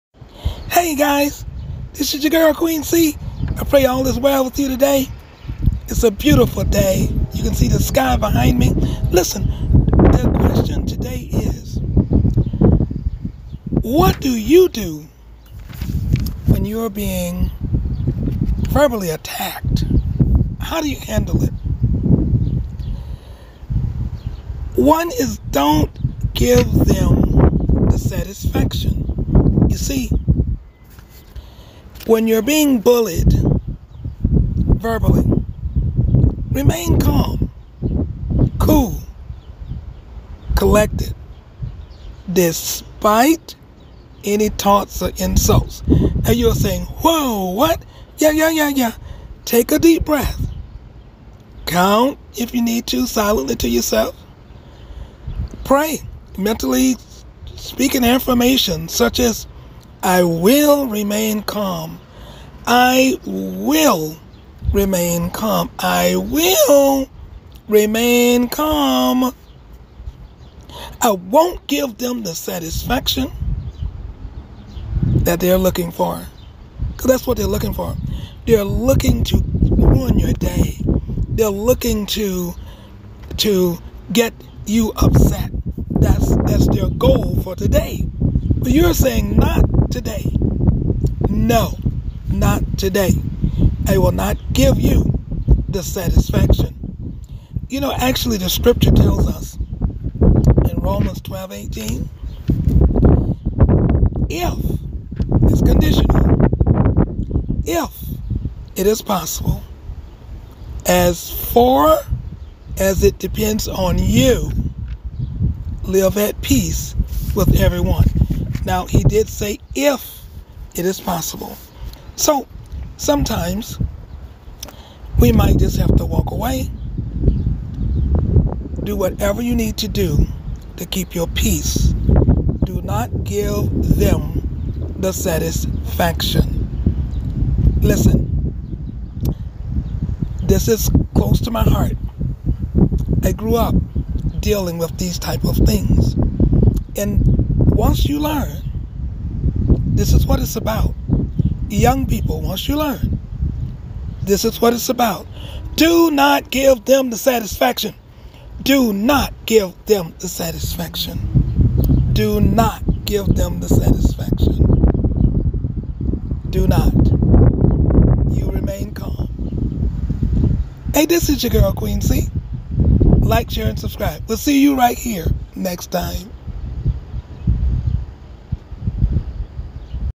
When You Are Verbally Attacked?
Remain Calm, If Possible
 The best way to deal with an abusive person is to remain calm. Since being upset (or even emotional) can escalate the situation. If you aren't sure how to stay calm, you can take deep breaths when engaging with this person to calm you down before you speak.